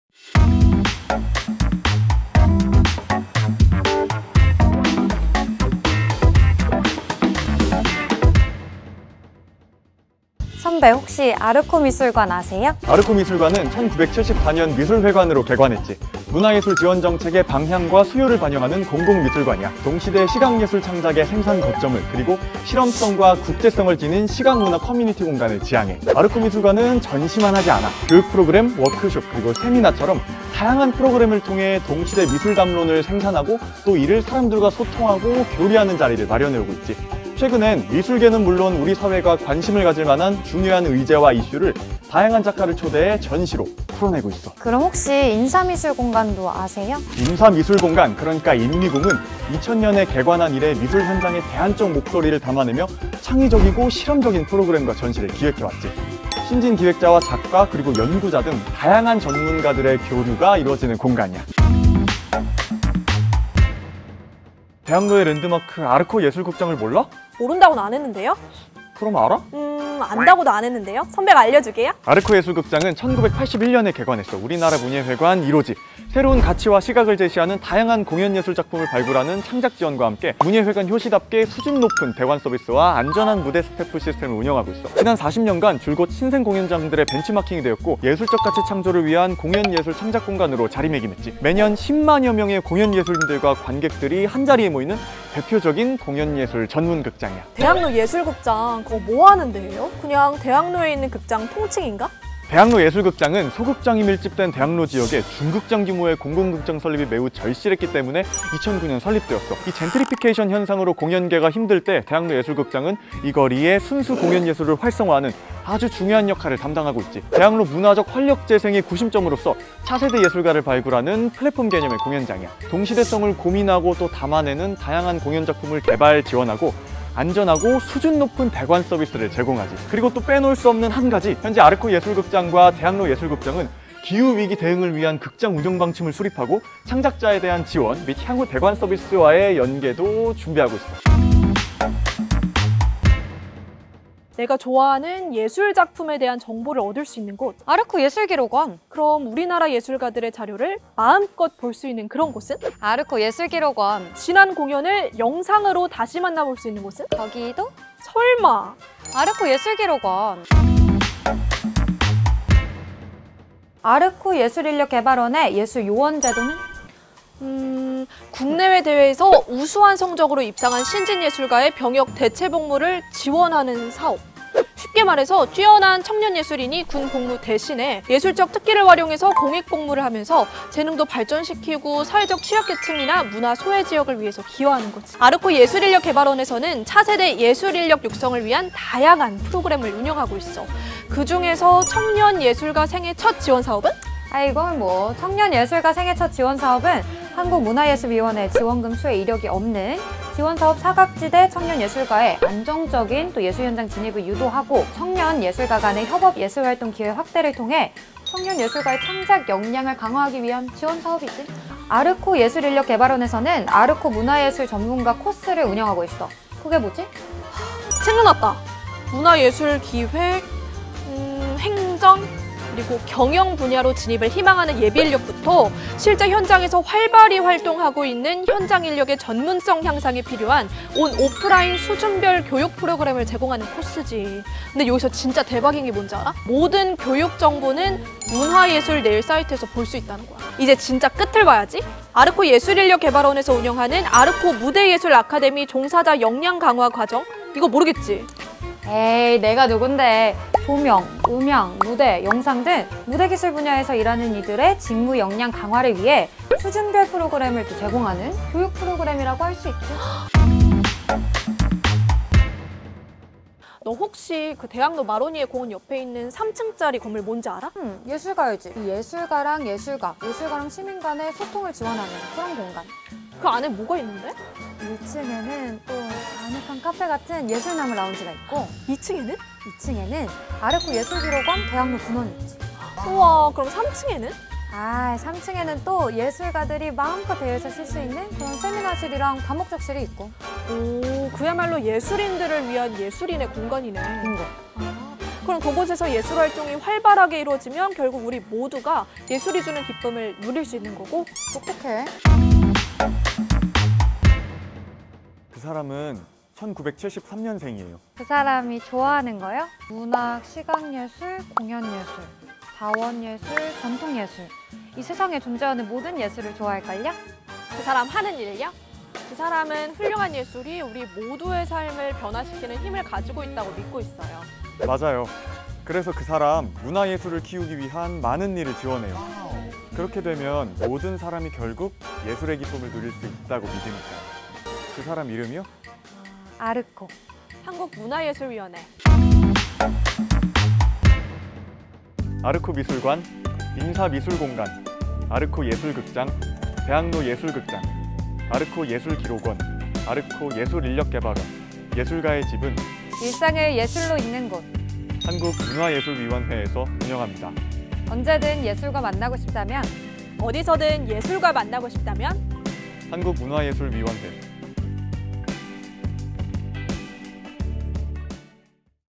[한국문화예술위원회] MZ, 아르코를 만나다 - 일상을 예술로 잇는 곳
아르코미술관, 인사미술공간, 아르코예술극장, 대학로예술극장, 아르코예술기록원, 아르코예술인력개발원, 예술가의집은 모두 50년 역사를 간직한 한국문화예술위원회에서 운영합니다.

언제든 예술과 만나고 싶다면
어디서든 예술과 만나고 싶다면
 '일상을 예술로 잇는 곳'  - 한국문화예술위원회(아르코)